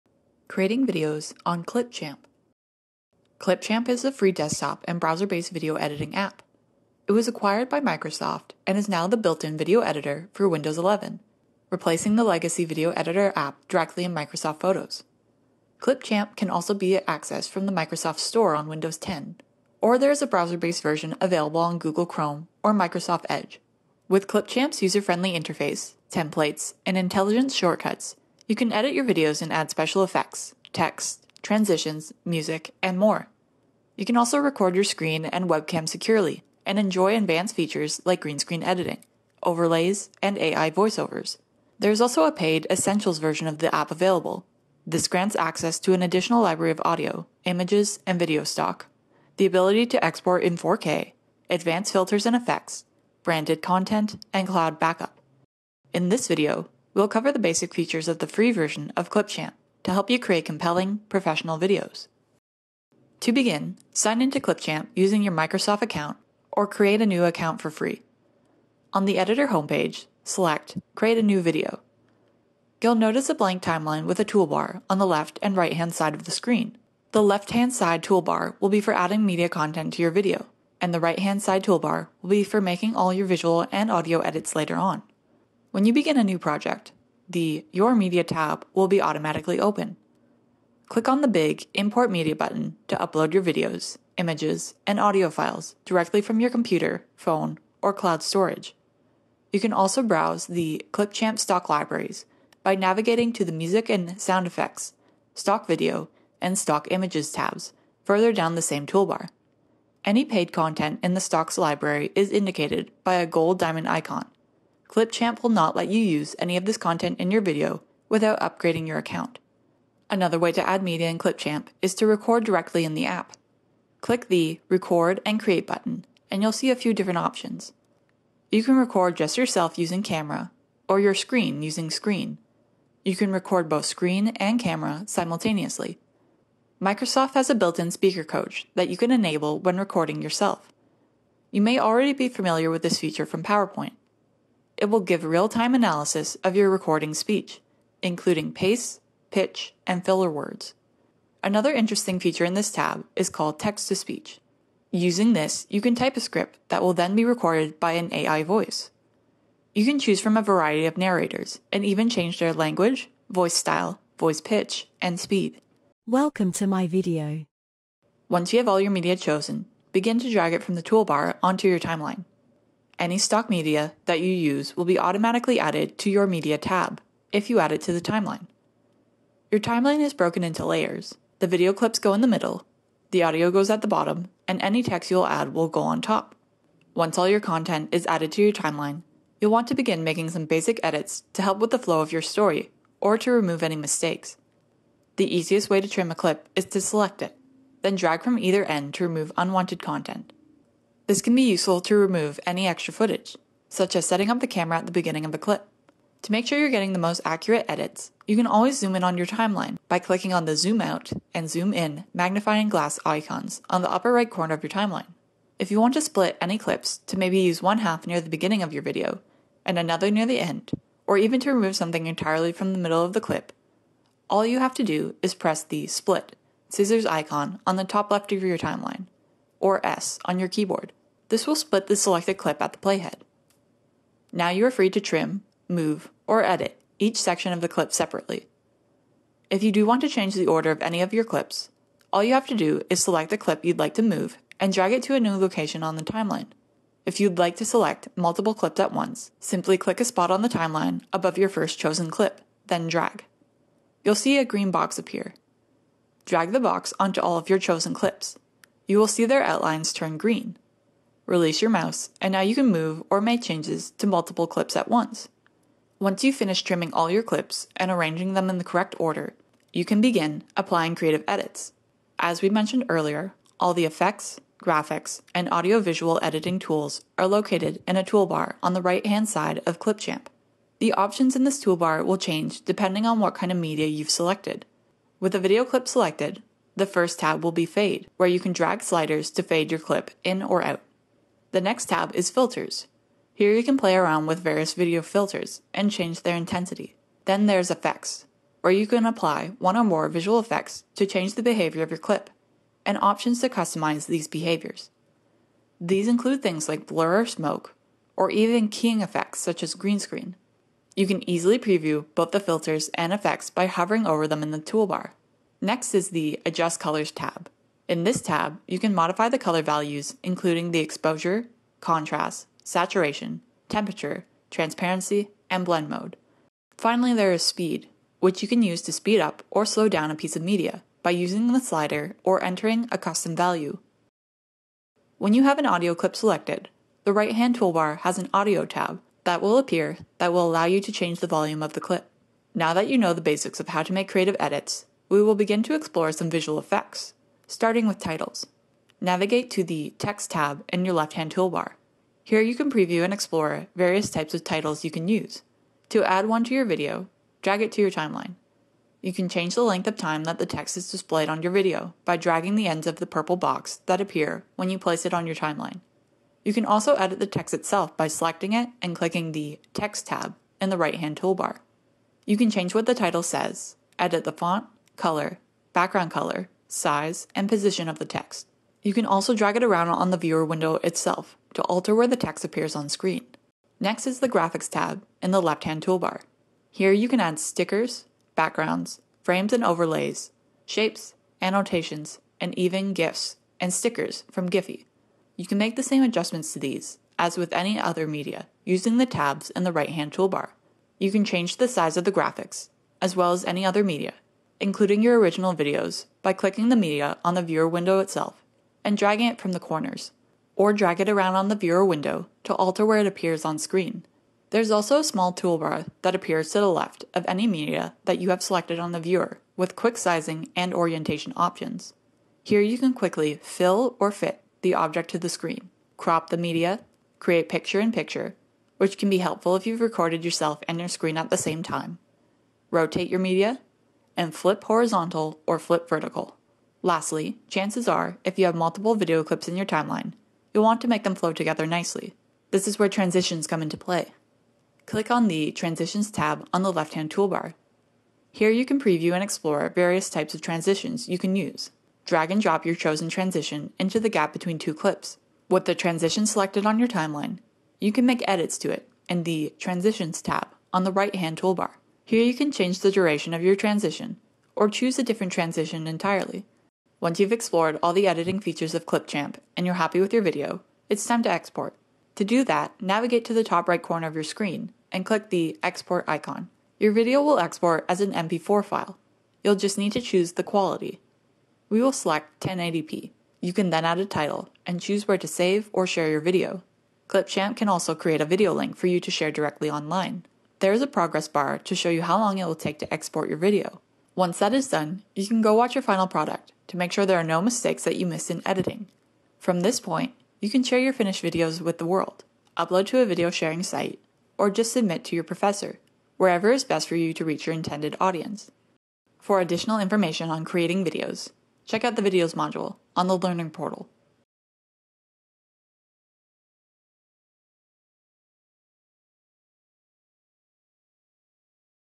Creating Videos Using Clipchamp
0:00 Introduction
1:05 Opening Clipchamp
1:25 Importing
1:59 Recording in app
2:31 Text to Speech
2:48 Timeline
3:14 Editing clips
4:25 Effects and filters
5:35 Audio
5:47 Text
6:23 Graphics
7:16 Transitions
7:46 Exporting